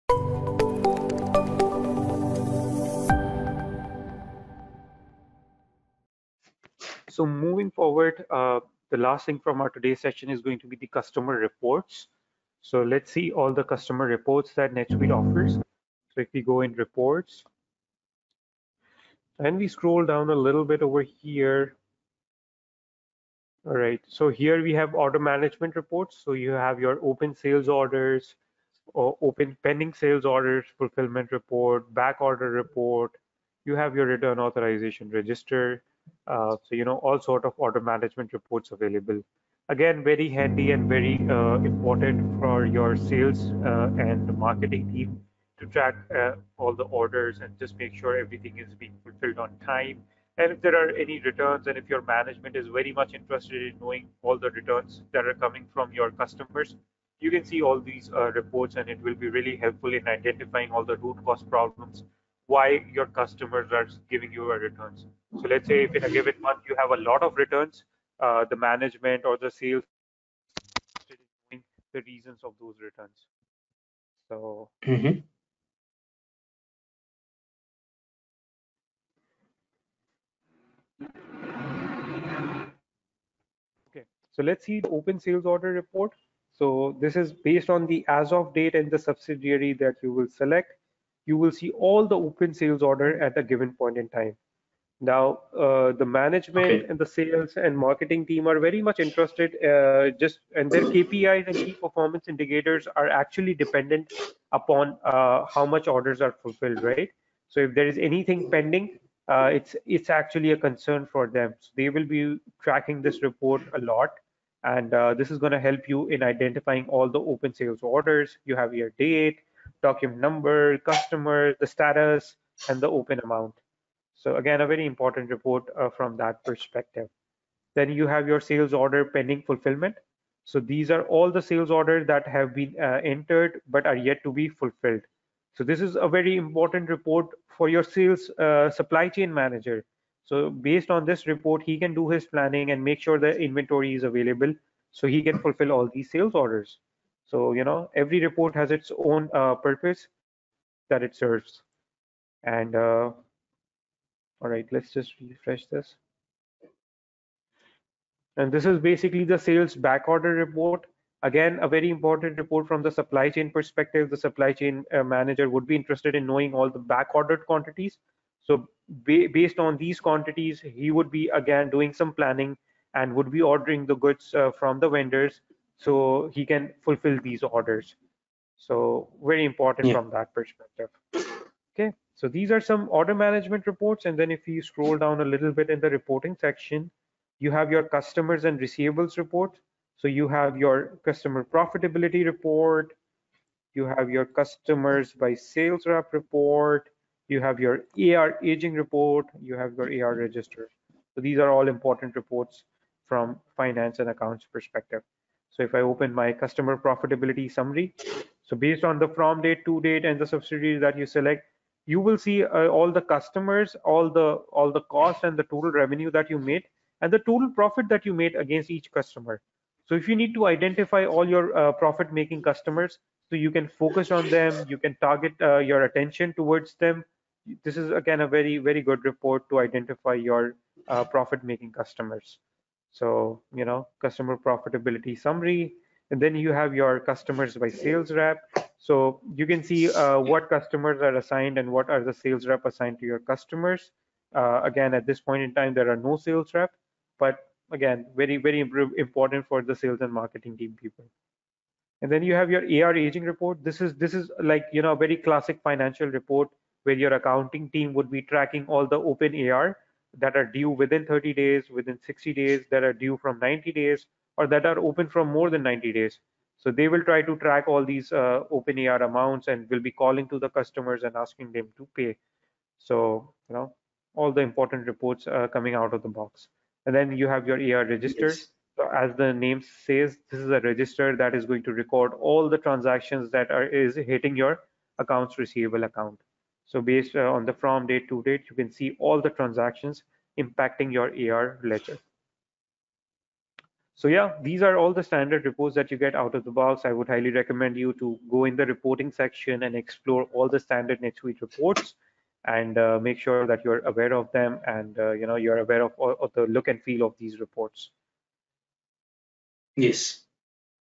NetSuite Tutorials | Oracle Netsuite Training | NetSuite Training for Beginners | Upptalk
NetSuite is a powerful, cloud-based business management software suite that offers a comprehensive set of tools for managing finances, customers, operations, and more. You'll learn how to use NetSuite to streamline your business operations, save time and money, and improve your bottom line.
In this video you will know about Customer reports of Sales and Order in NetSuite.